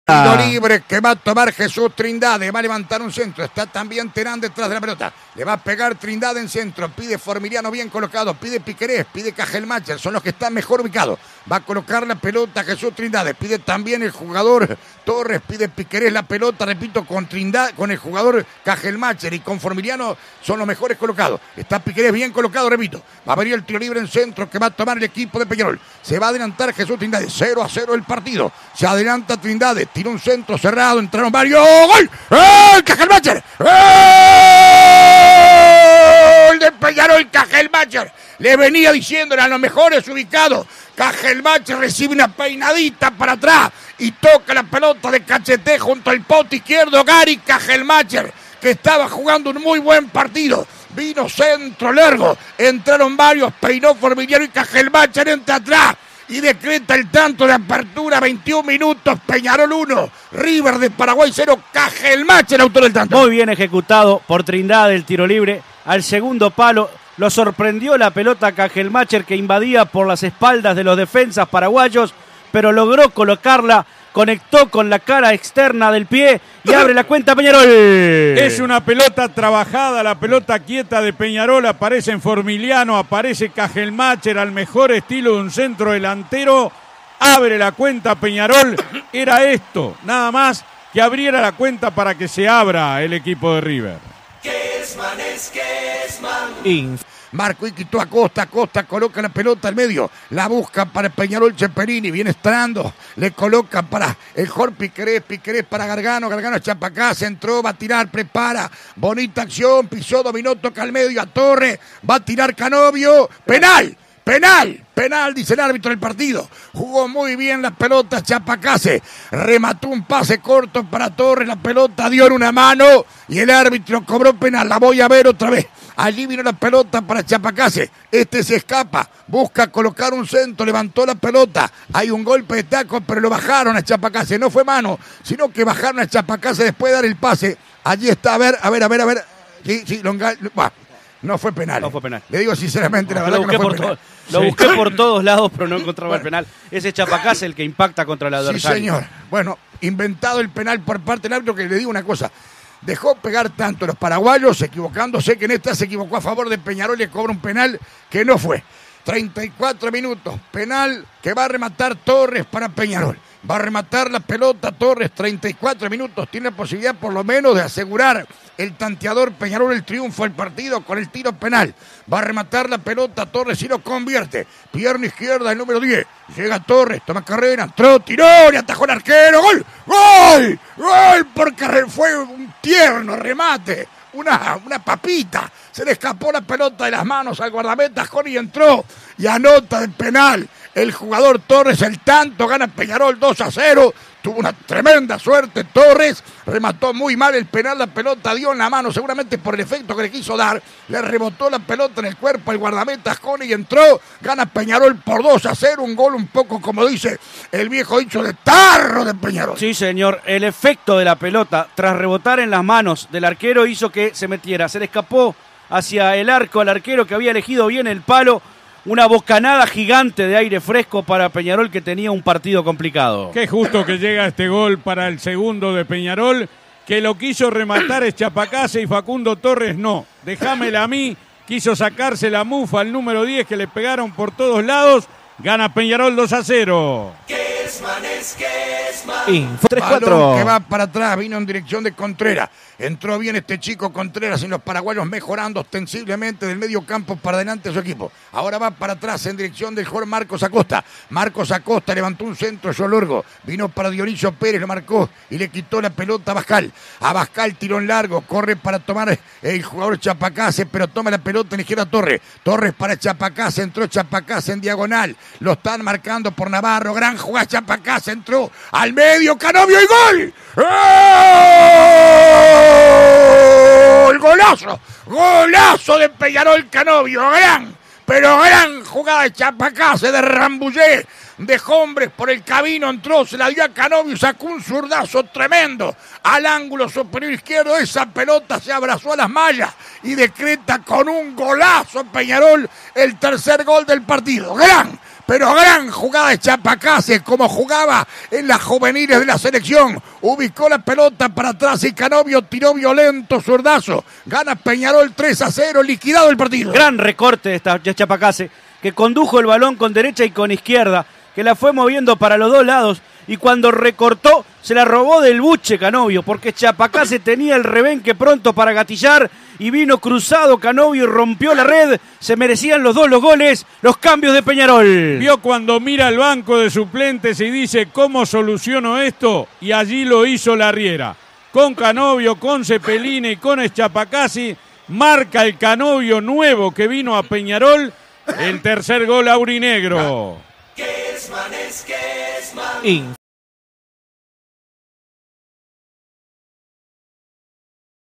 Peñarol 3 River Plate [PY] 0 - Sudamericana 2021 (por Alberto Kesman)
Los goles del partido, por Alberto Kesman y equipo para CX22 Radio Universal 970 AM.